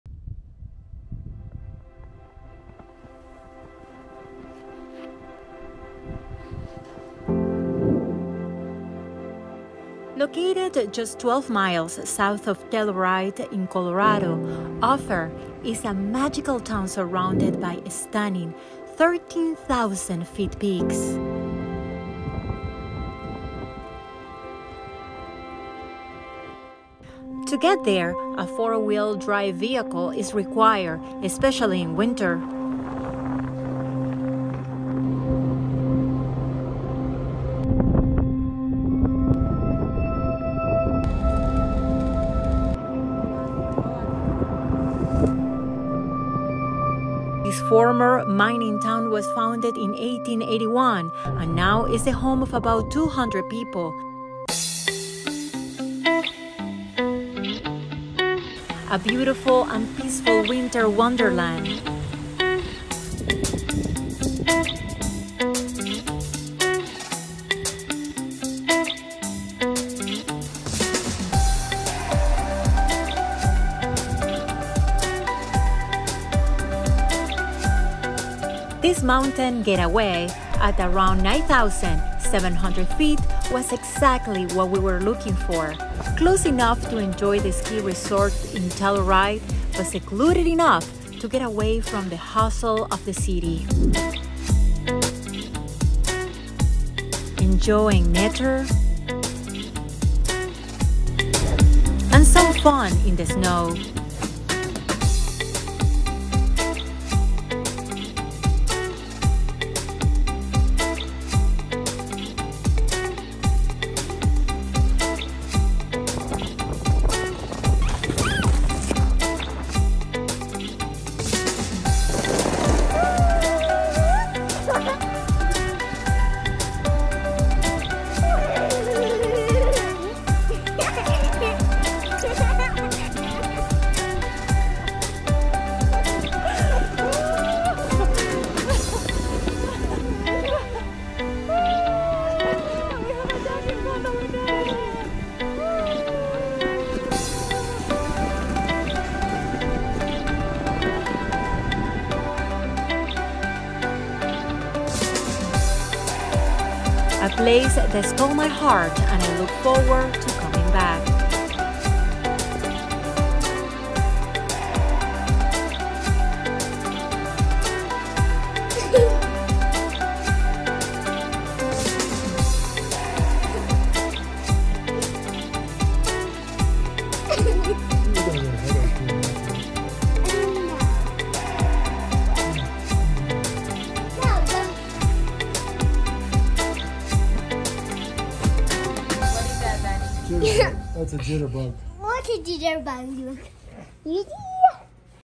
WINTER IN OPHIR, COLORADO
Ophir, Colorado is a former mining town founded in 1881. Now it is the home of about 200 people, many of them outdoor lovers. Located only 12 miles from Telluride, this town was a perfect combination: close enough to the ski resort, but secluded enough among gigantic peaks away from the bustle of the city. Enjoy this magical winter wonderland!